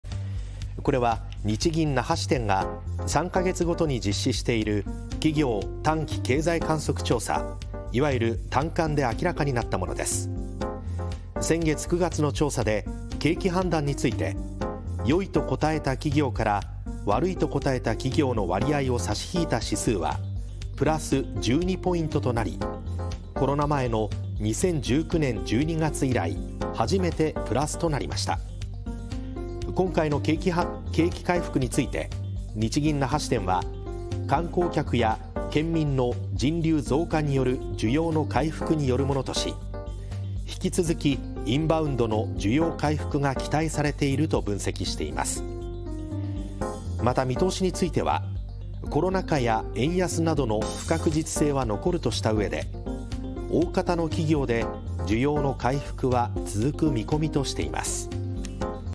コロナ後“初”となる県内景気が回復 要因は「人流増加で需要回復」 日銀短観
日銀那覇支店が3日に発表した県内の景気判断を示す指数がコロナ前の2019年12月以来、初めてプラスとなりました。2022/10/06